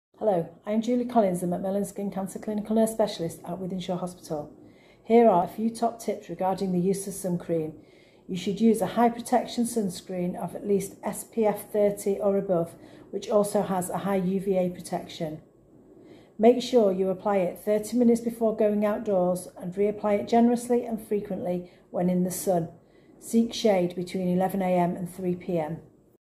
Skin cancer nurse Julie talks about staying safe in the sun
Skin cancer specialist nurse Julie talks about how to keep your skin protected in the sun.